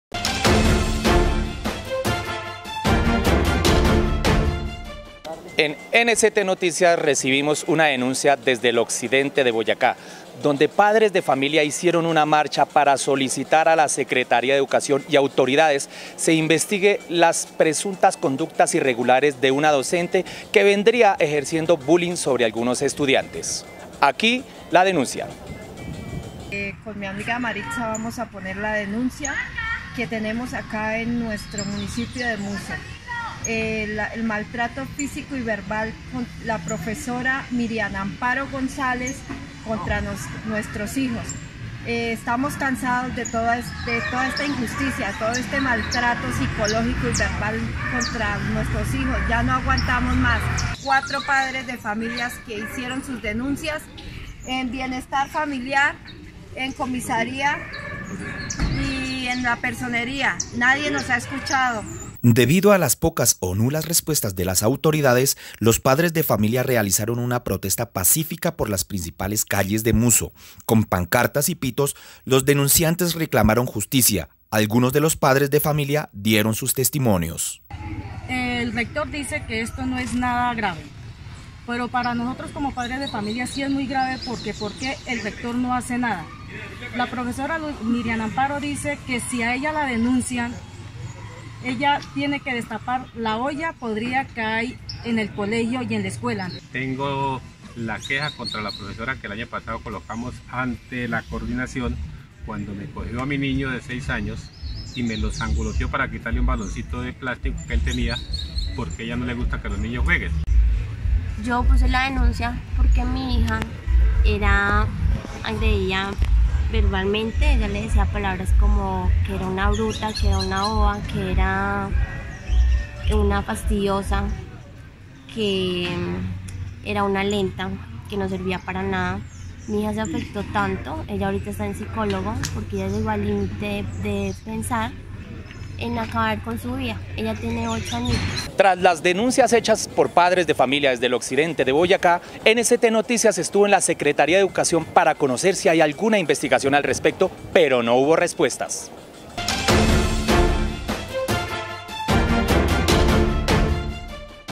DenunciaGeneralNct: Con pitos y pancartas, varios padres de familia
DenunciaGeneralNct: Con pitos y pancartas, varios padres de familia realizaron una protesta en Muzo exigiendo que autoridades investiguen los presuntos maltratos de una docente a varios niños.